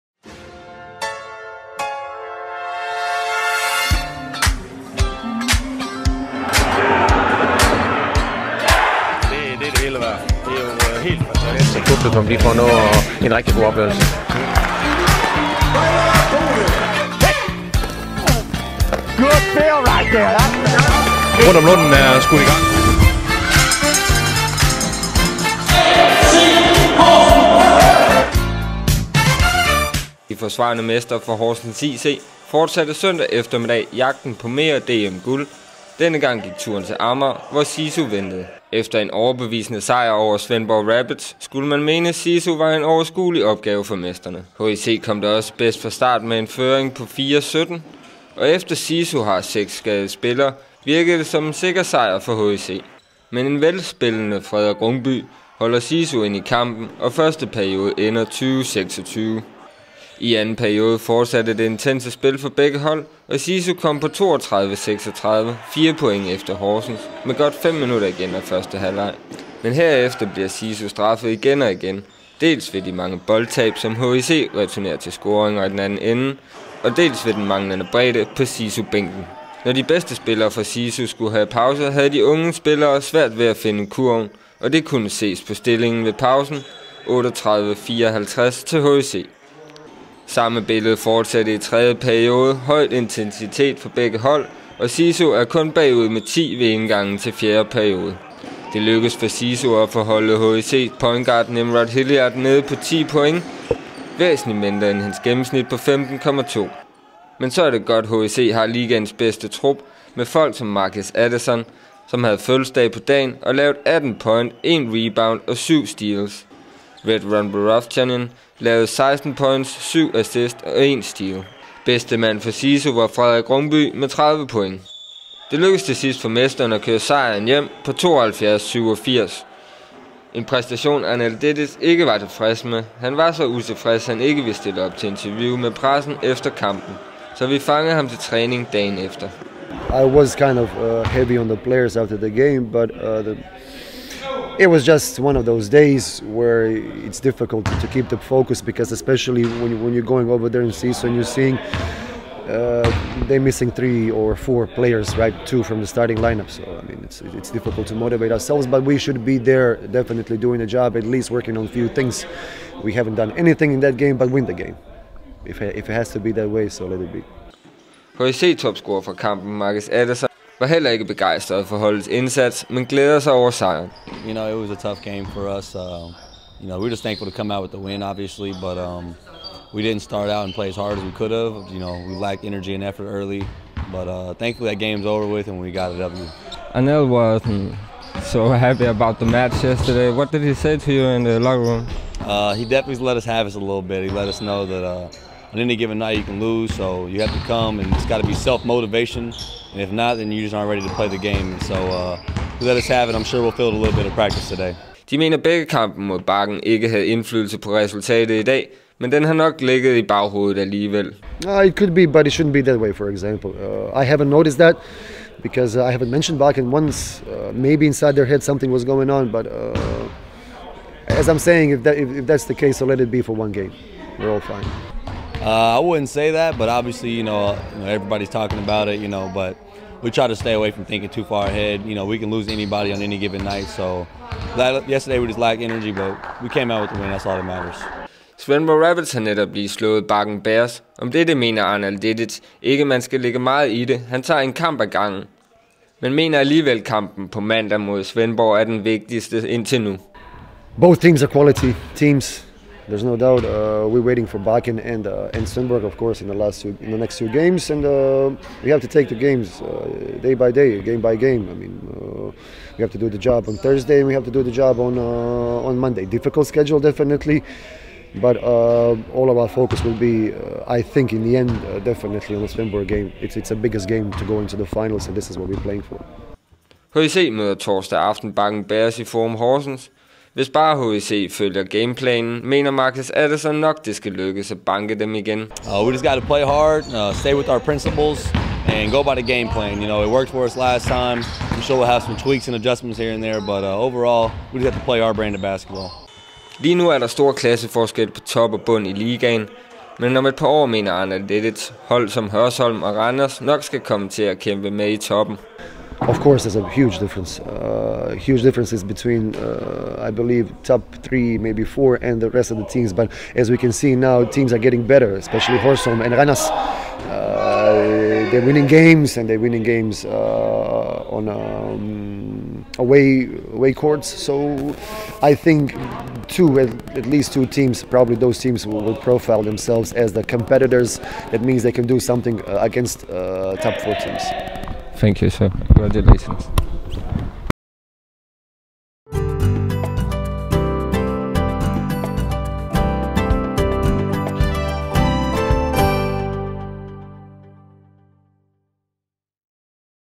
TV Horsens Sporten - SISU vs HIC
Søndag eftermiddag tog Horsens IC til Ammar for at møde SISU.
På papiret en overskuelig opgave men det blev det ikke for de forsvarende mestrer der måtte kæmpe for en sejer. Det lykkedes dog  til sidst at hive en sejer hjem på 72–87. Se Highlights og interview med Arnel Dedic & Marquis Addison her: